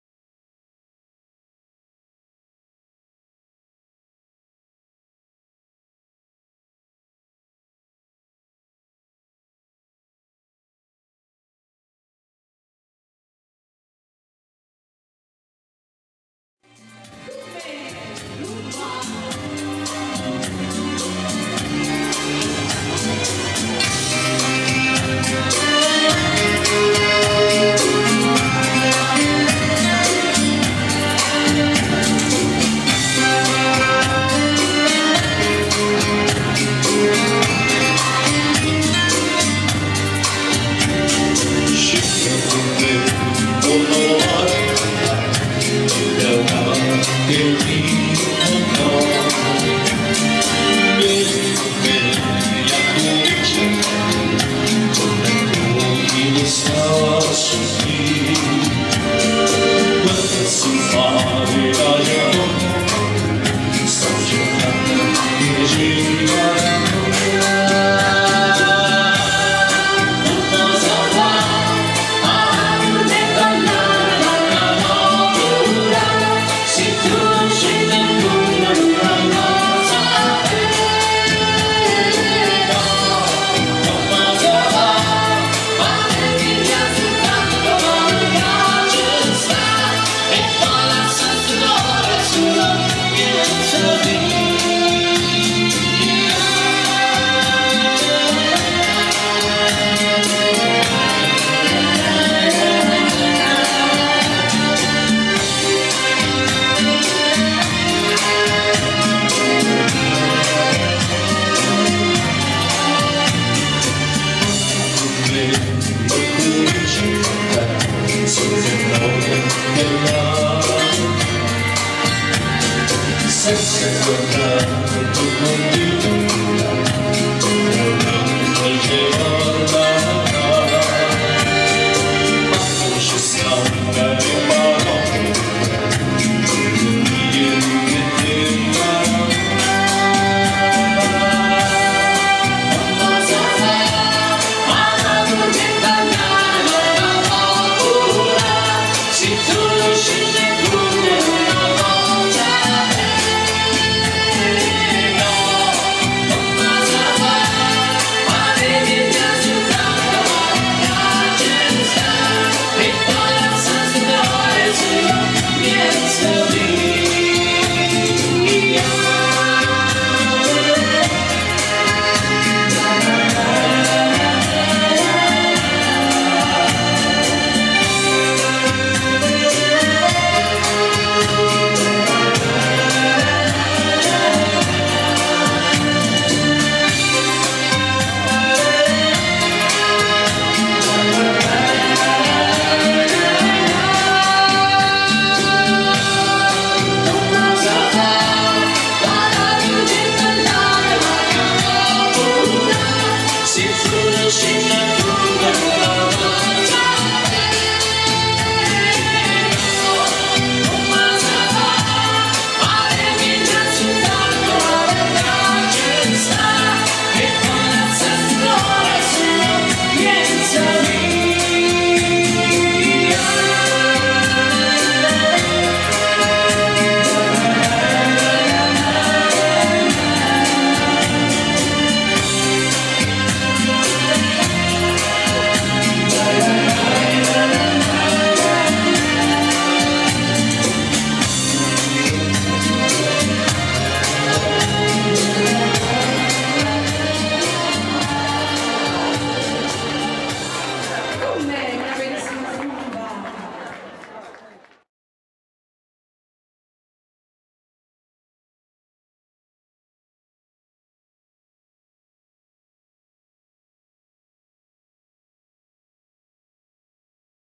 Rumba con il "Trio Italiano"
Una bella canzone che il "Trio Italiano" interpreta molto bene. I video che carico sono ripresi sempre dal vivo. e voi dal vivo potete commentarli. Buona serata a tutti!